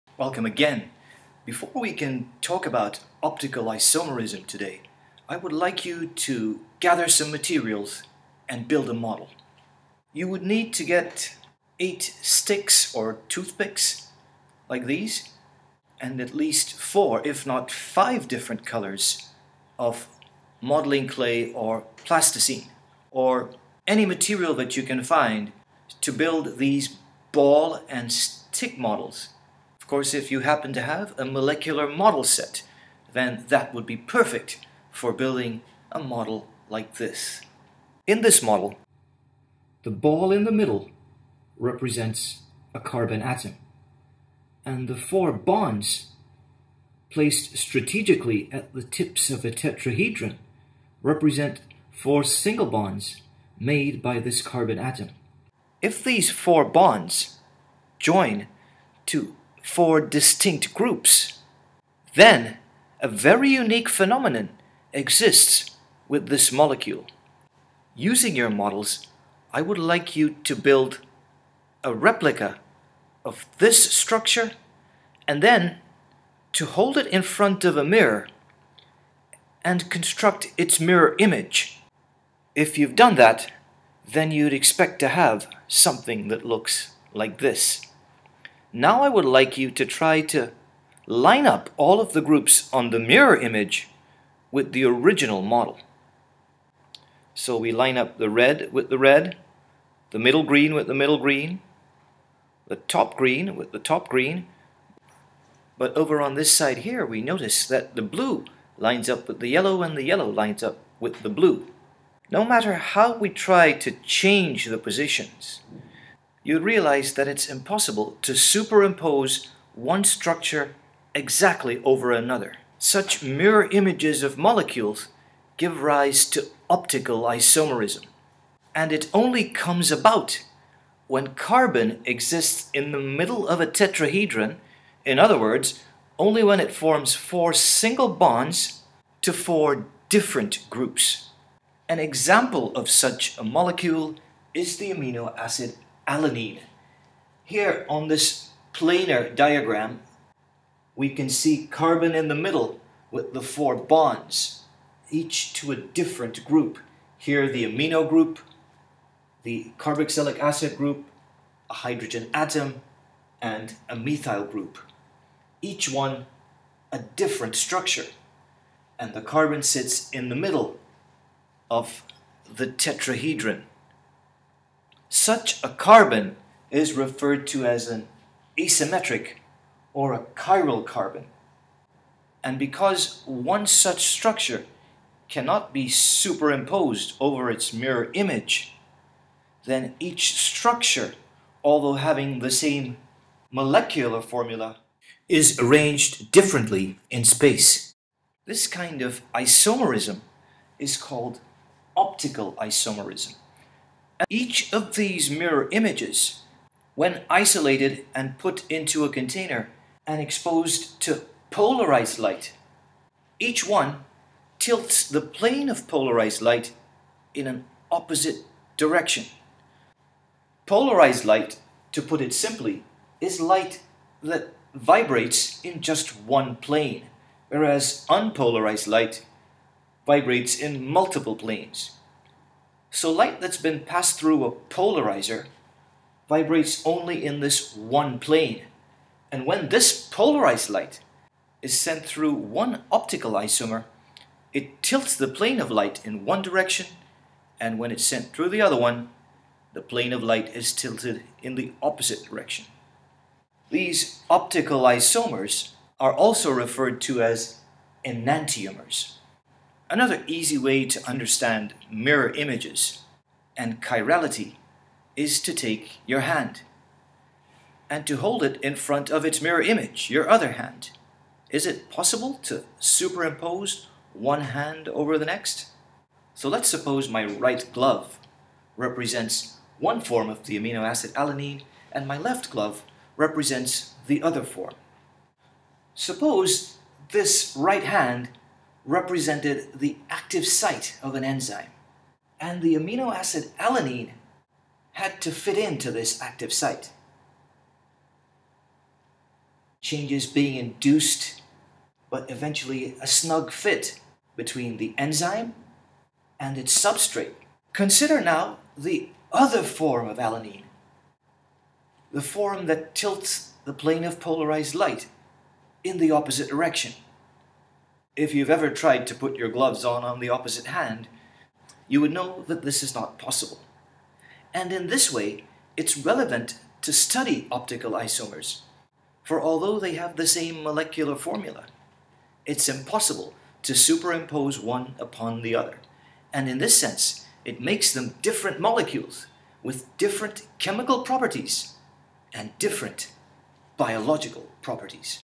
IB Chemistry-Optical Isomerism
Dubay demonstrates chirality and explains the significance of optical isomerism. It is useful if you build a model yourself as recommended in the video lesson. This lesson focuses on a single chiral center in a carbon atom and is targeted at students of introductory biology and IB Chemistry HL.